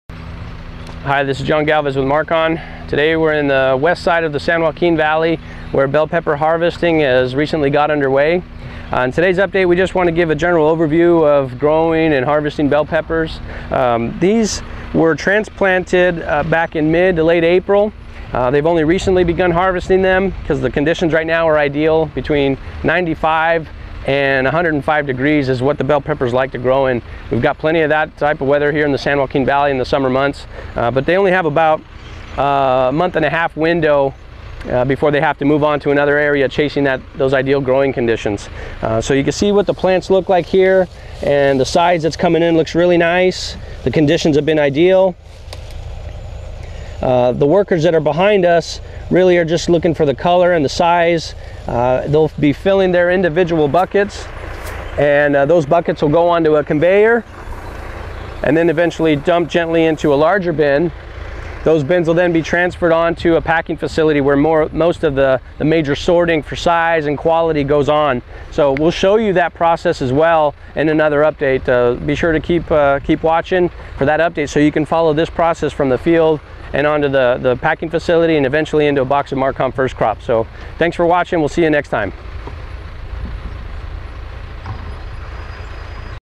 Markon Live from the Fields, Green Bell Pepper Harvest, July 30, 2012, Huron, California
John Galvez, Markon's quality assurance director, reports live from a bell pepper field in Huron, California in this two-part video. In part one, John highlights green bell pepper harvesting. Growing conditions are favorable and Markon First Crop Green Bell Peppers are available.